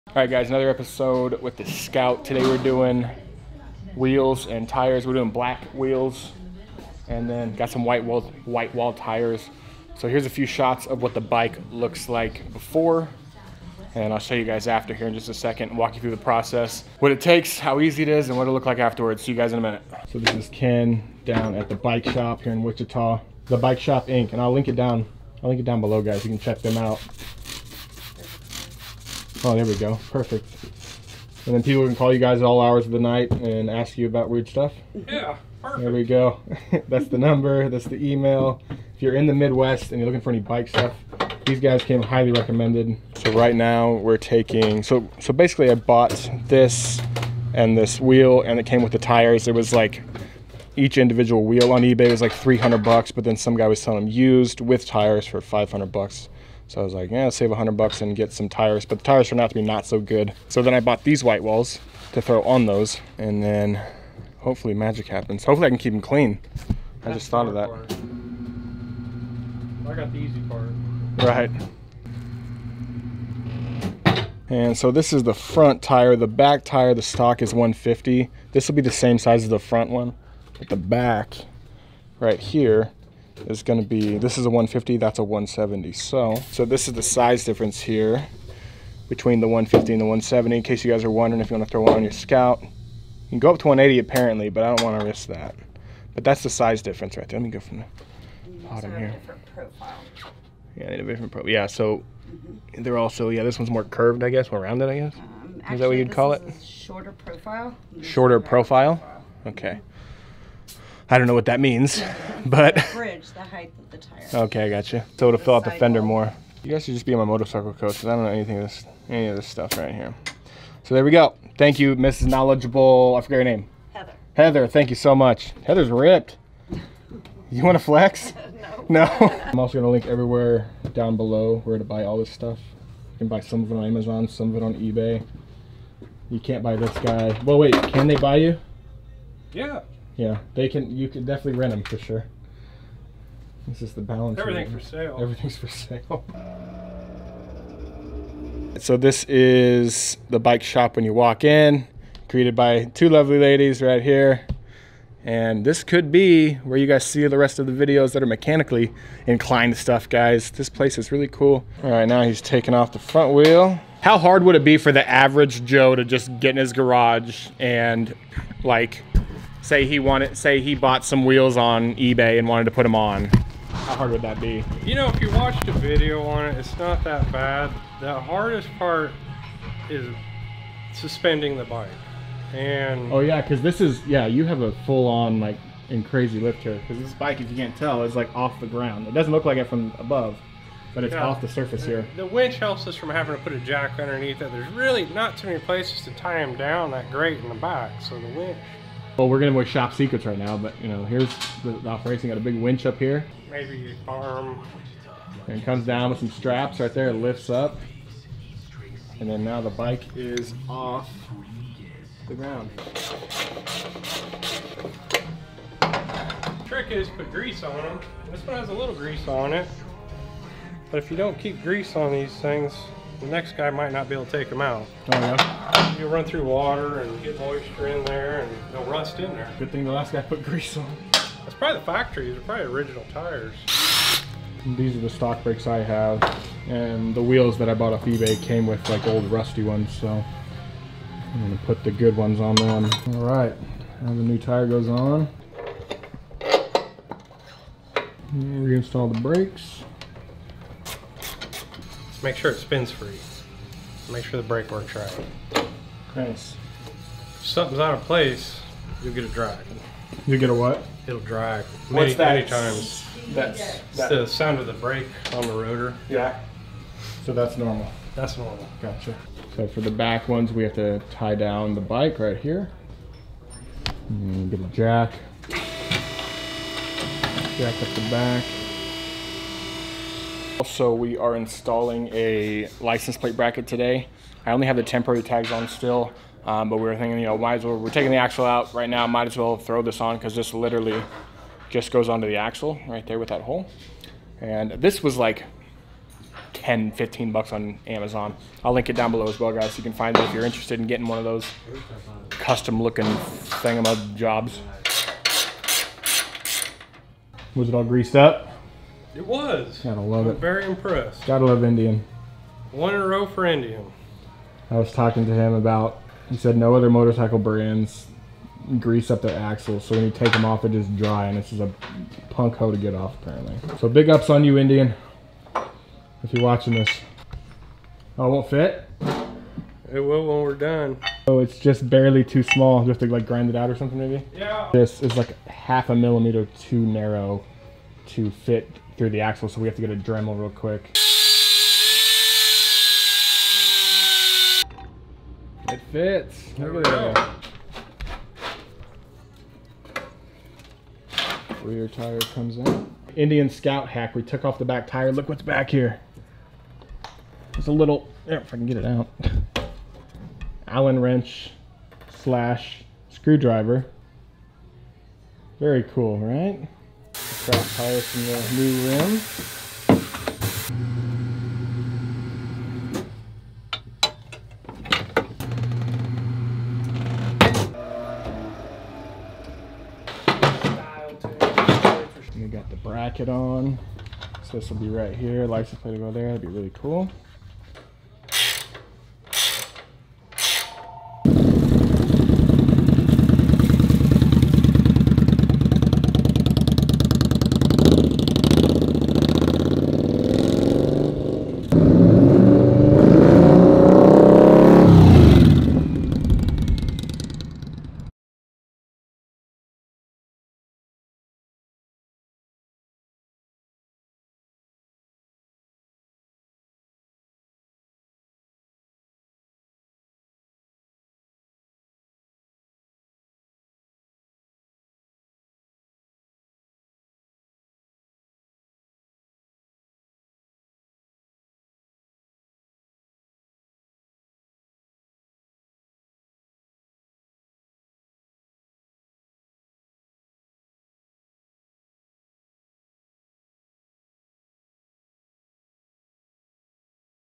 170 Rear Shinko Tire Install and New Wheels | Indian Scout Project | Episode 8
Rear Tire
Front Tire
License Plate Bracket
Today I install some shinko white wall tires and custom side licenses plate bracket on my 2016 indian scout.  Check out the project!